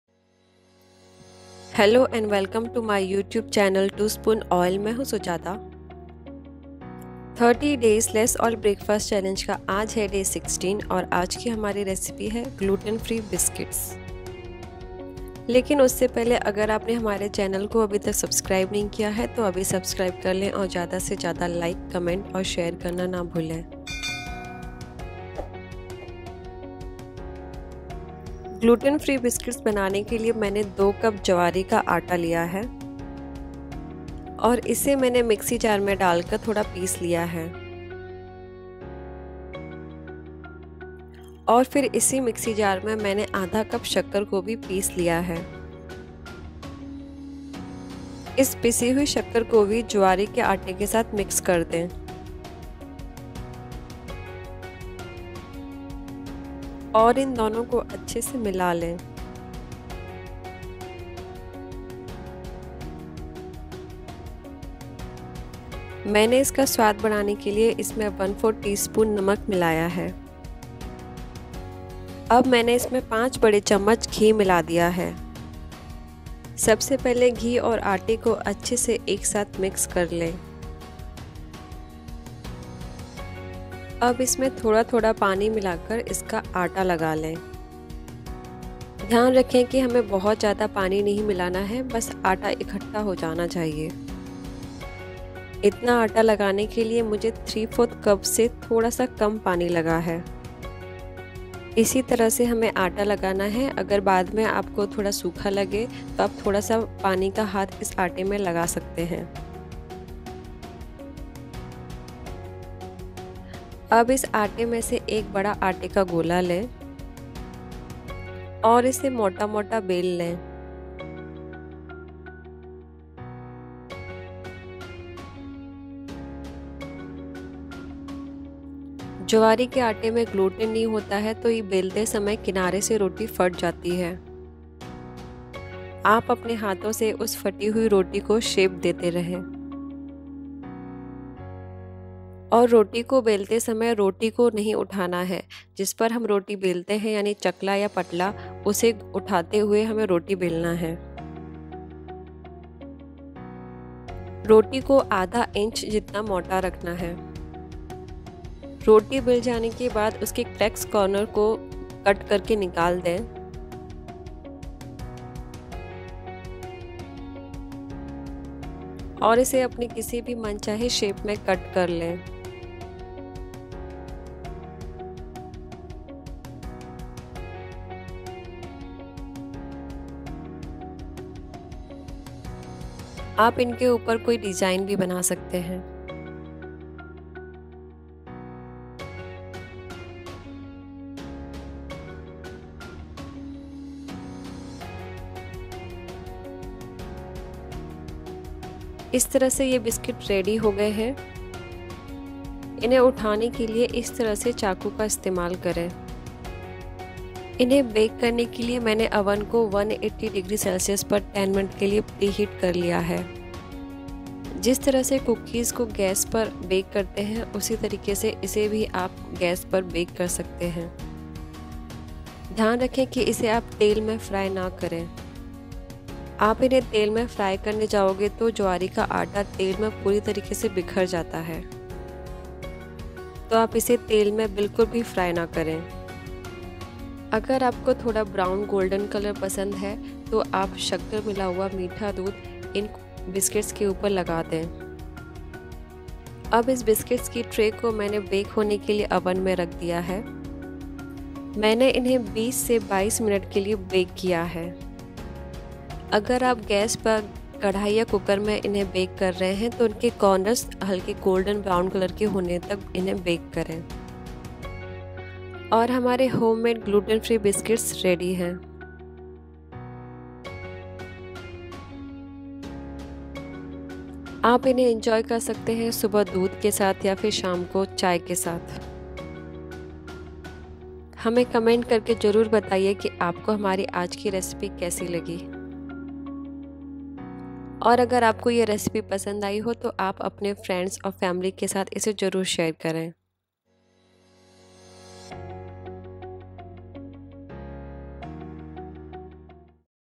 GLUTEN FREE BISCUIT | 30 days less oil breakfast recipes | Healthy recipes
Welcome back to two_spoon_oil by Sujata.

INGREDIENTS :
 JOWAR / SORGHUM FLOUR - 2 CUP